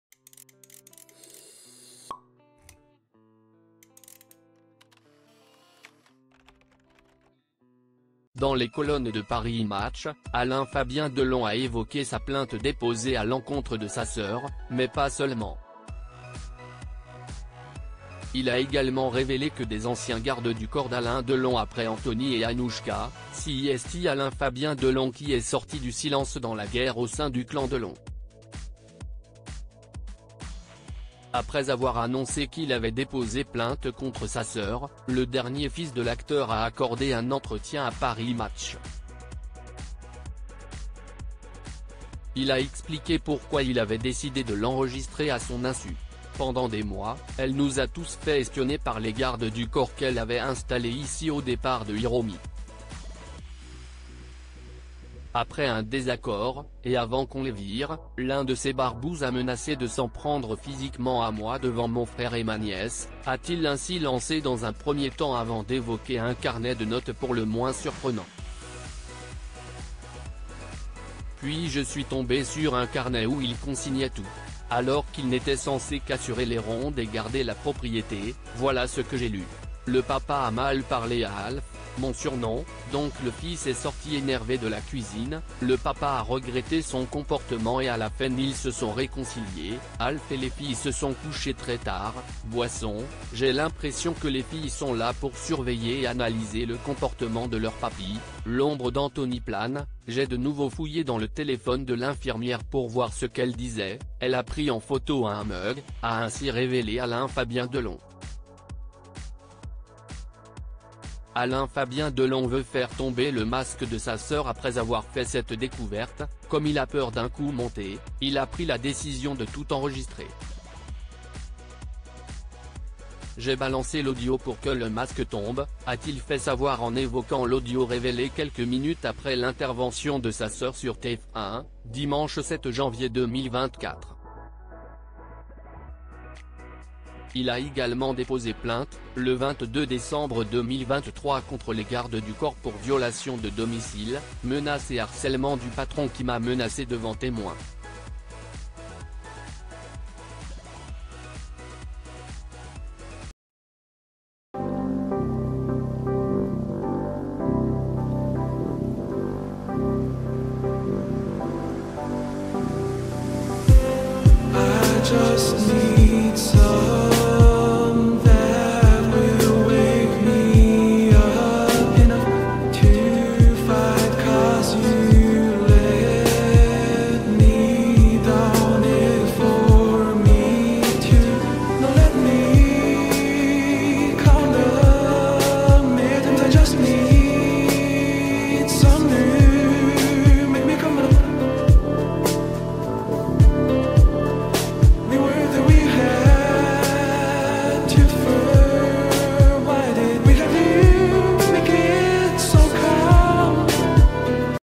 Affaire Alain Delon ces notes hallucinantes prises par le garde du corps de l’acteur selon Alain F
Affaire Alain Delon : ces notes hallucinantes prises par le garde du corps de l’acteur selon Alain-Fabien